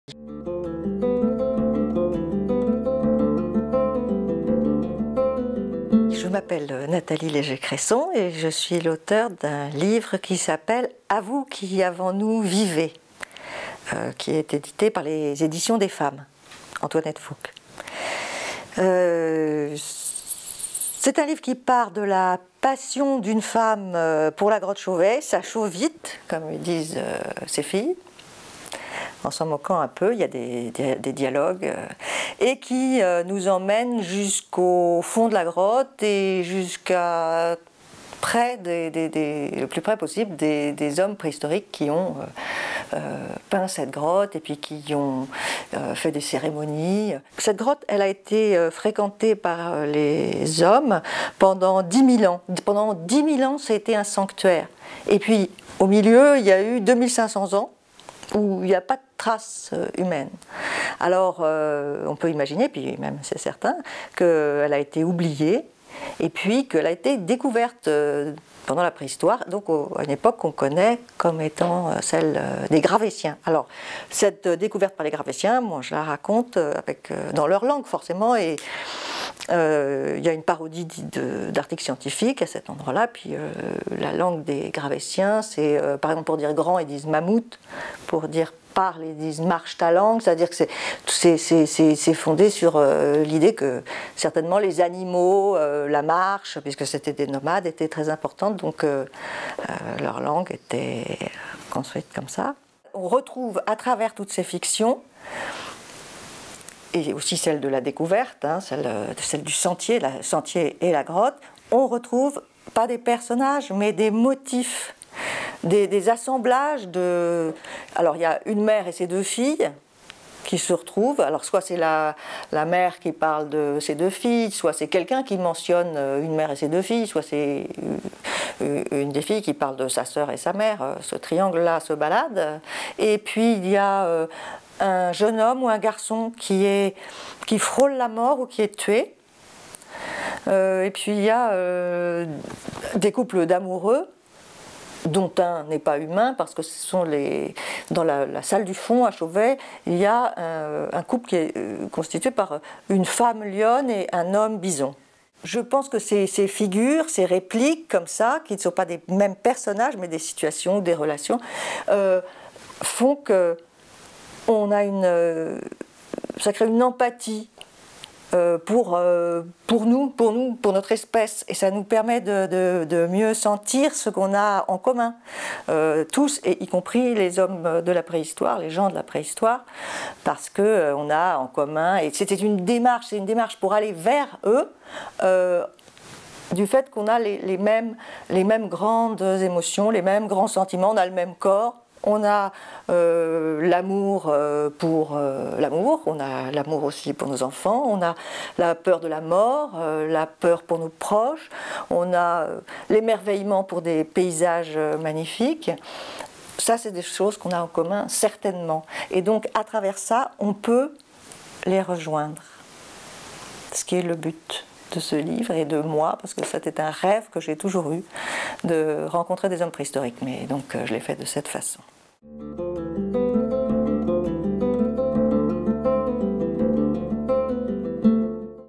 Nathalie Léger Cresson - A vous qui avant nous vivez
Nathalie Léger Cresson vous présente son ouvrage "A vous qui avant nous vivez" aux éditions Des femmes-Antoinette Fouque.